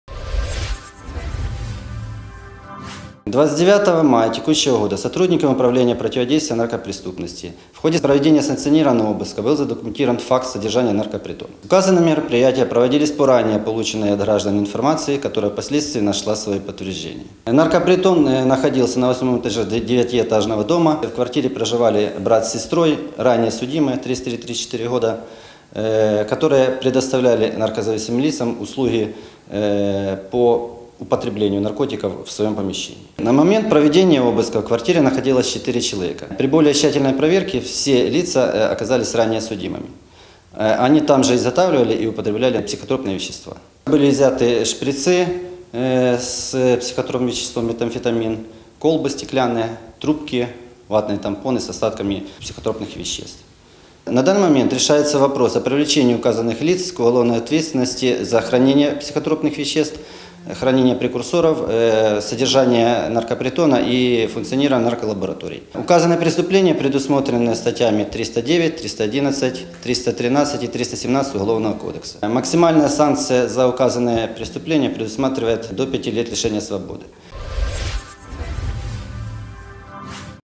Викриття наркопритону у м. Дніпро
Завдяки оперативному реагуванню на скаргу громадян правоохоронці «накрили» наркопритон у житловій багатоповерхівці.

Громадяни поскаржилися до відділення поліції, що мешканці однієї з квартир на житловому масиві Перемога м. Дніпра ведуть аморальний спосіб життя і до них постійно приходять підозрілі люди.

В результаті в ході проведення обшуку поліцейські задокументували діяльність раніше неодноразово судимих брата і сестри, які влаштували в квартирі наркопритон. Було встановлено, що власники вказаної квартири систематично надавали наркозалежним особам своє житло для вживання наркотиків.

29 травня працівники управління протидії наркозлочинності ГУНП в Дніпропетровській області провели санкціонований обшук в рамках кримінального провадження, відкритого за ч. 1 ст. 307 Кримінального кодексу України. Під час обшуку були виявлені та вилучені шприци з психотропною речовиною «метамфетамін», обладнання та прекурсори для виготовлення наркотику – скляні колби, скляна трубка, шприци. На момент проведення слідчих дій в квартирі знаходилися двоє раніше судимих чоловіків, які вживали метамфетамін. Їх доставили до наркодиспансеру для огляду лікарем-наркологом.

Всі вилучені речовини та предмети направлені на дослідження. Наразі вирішується питання про внесення відомостей до ЄРДР за ознаками кримінального правопорушення, передбаченого статтями 309, 311, 313, 317 КК України. Затриманим загрожує до 5 років позбавлення волі.

Відділ комунікації поліції Дніпропетровської області